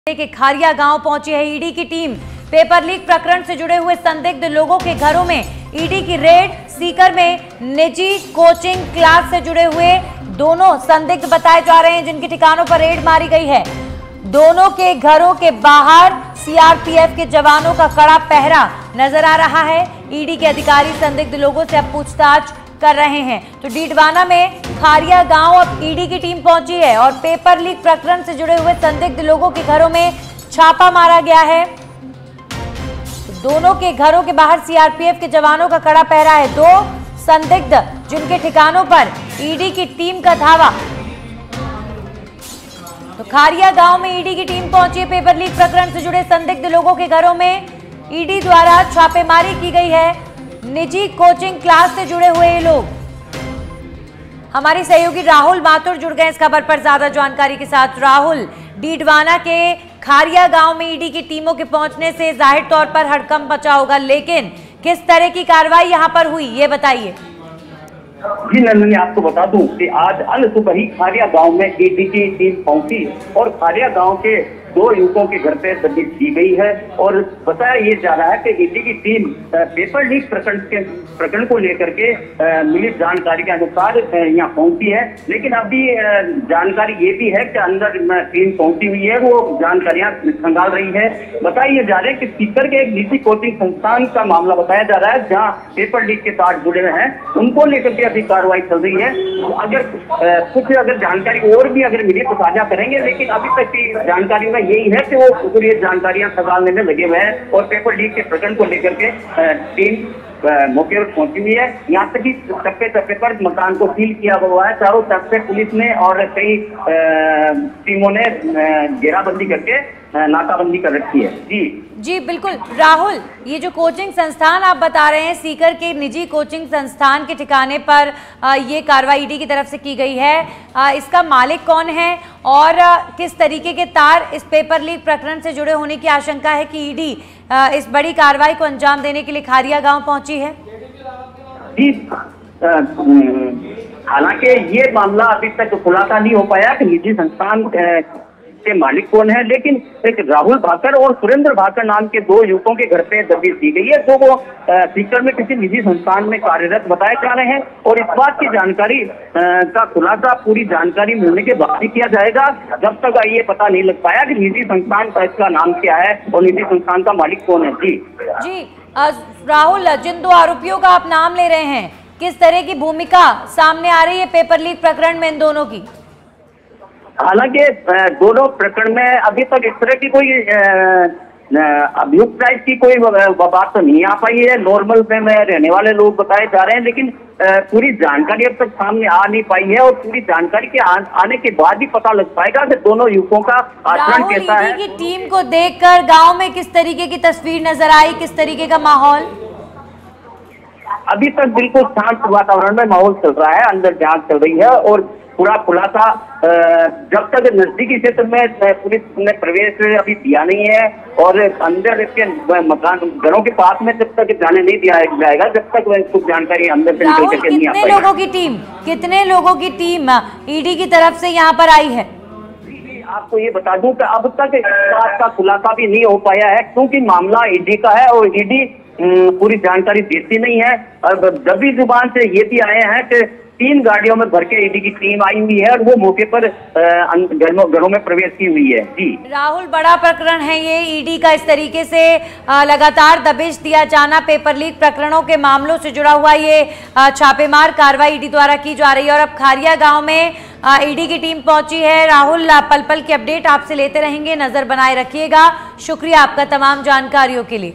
पेपरलीक प्रकरण से जुड़े संदिग्ध लोगों के घरों में ED की रेड | Didwana News | ED Raid News
About Video: पेपरलीक प्रकरण से जुड़े संदिग्ध लोगों के घरों में ED की रेड | Didwana News | ED Raid News